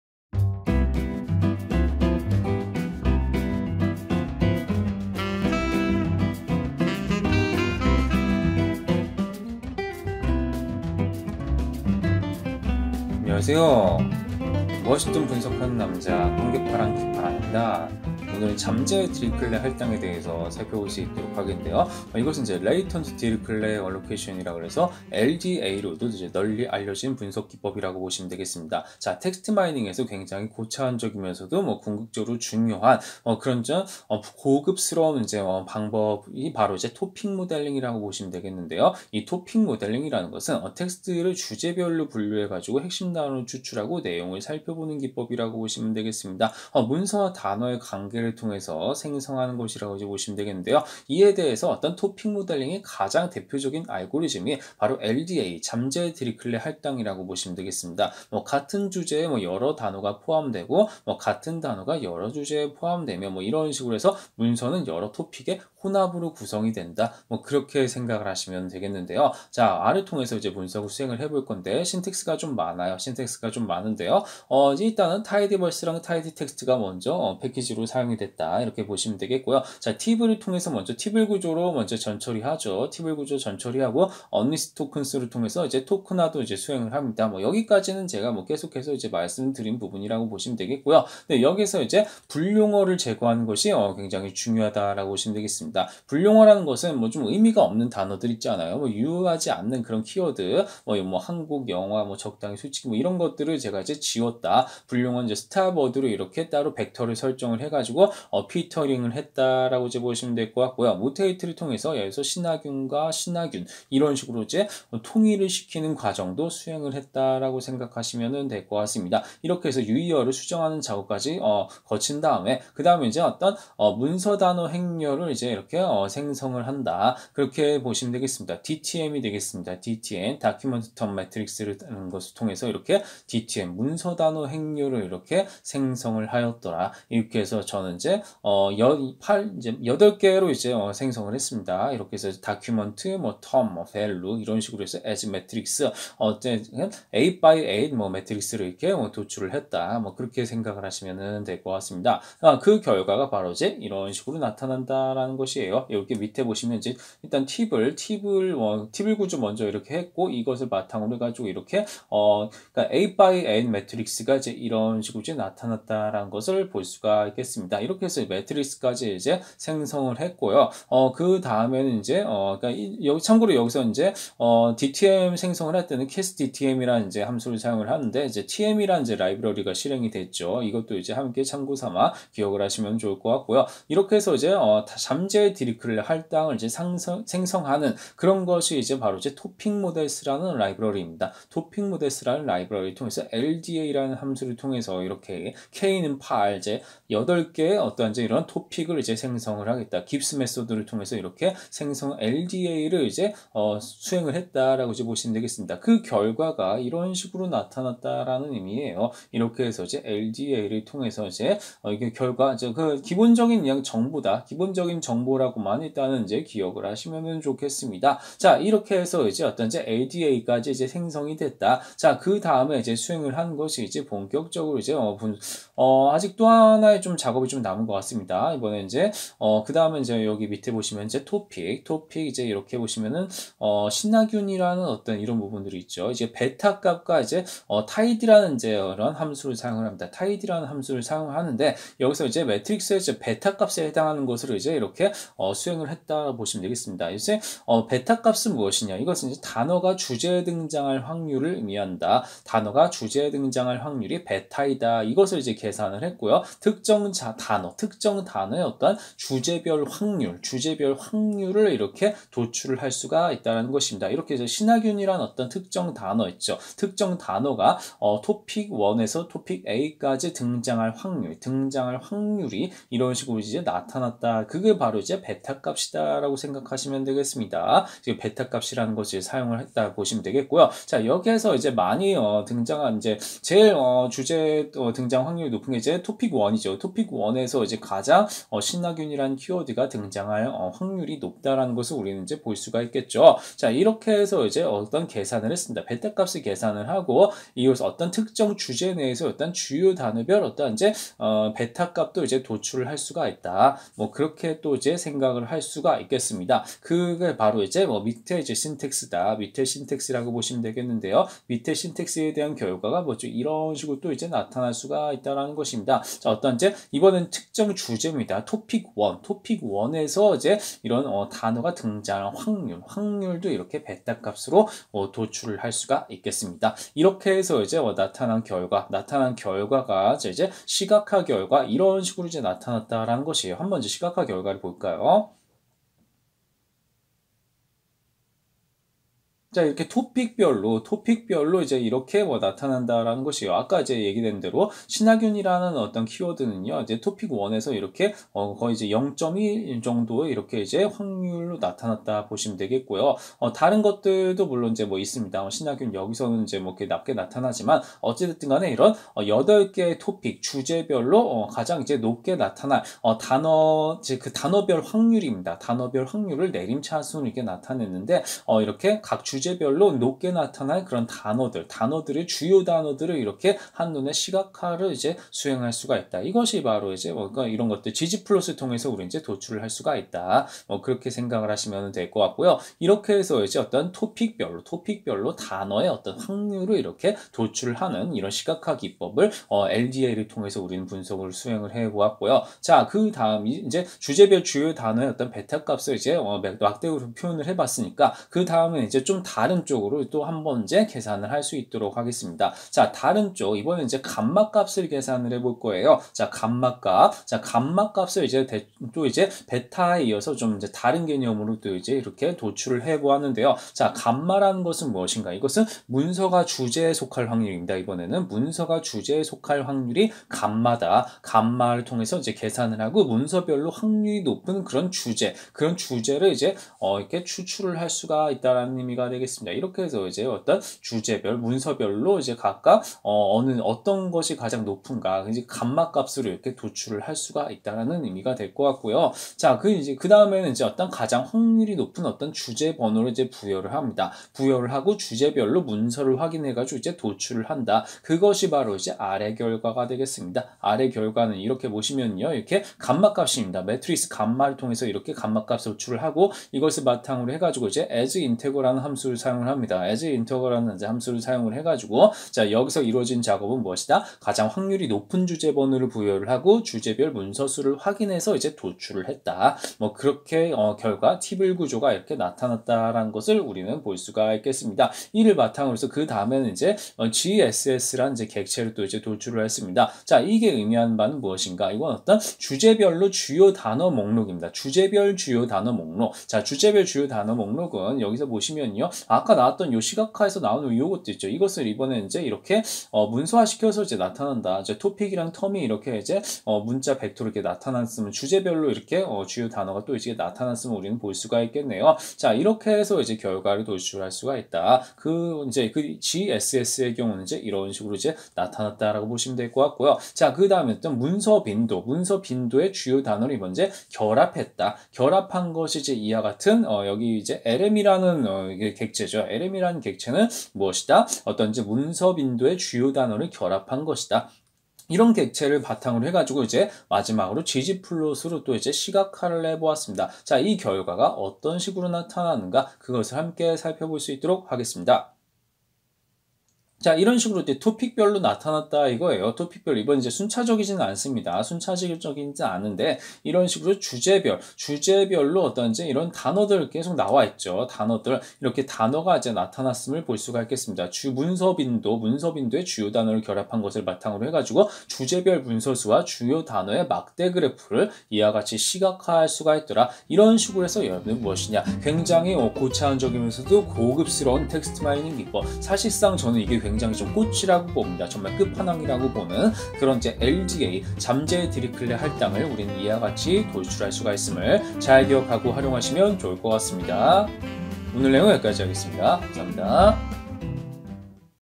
R을 활용한 텍스트마이닝 - (18) 토픽 모델링/잠재 디리클레 할당(LDA) (Topic Modeling)/(Latent Dirichlet Allocation)
토픽 모델링(Topic Modeling)
잠재 디리클레 할당(LDA)(Latent Dirichlet Allocation)

텍스트를 주제별로 분류하여 핵심
단어를 추출하고 내용을 살펴보는데
문서와 단어의 관계를 통해 생성합니다.

같은 주제에 여러 단어가 포함되고,
같은 단어가 여러 주제에 포함되며,
문서는 여러 토픽의 혼합이 됩니다.